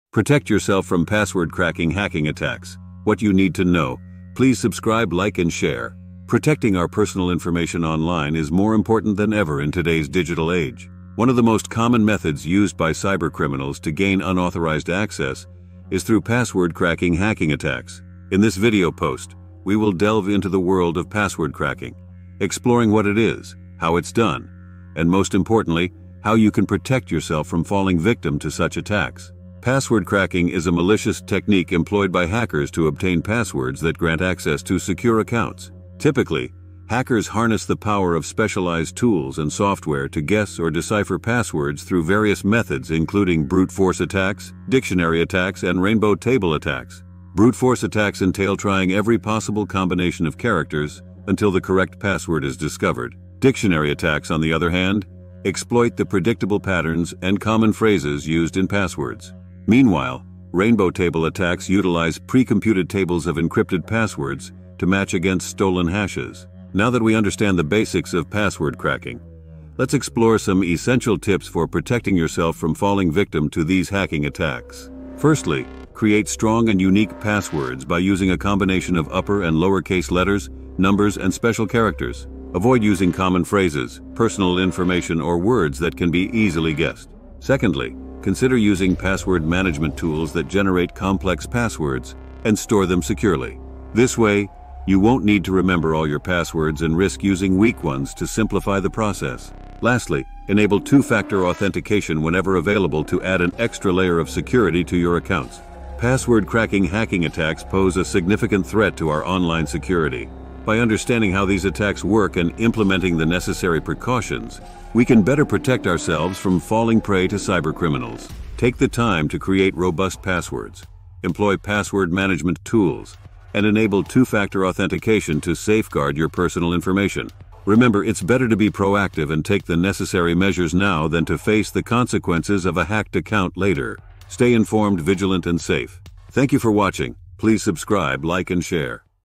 Protect Yourself from Password Cracking Hacking Attacks: What You Need to Know
Protecting our personal information online is more important than ever in today's digital age. One of the most common methods used by cybercriminals to gain unauthorized access is through password cracking hacking attacks. In this blog post, we will delve into the world of password cracking, exploring what it is, how it's done, and most importantly, how you can protect yourself from falling victim to such attacks.

Read more about Cybersecurity in my book: "Comprehensive Cybersecurity Handbook for Novice Readers: Introduction to Cybersecurity"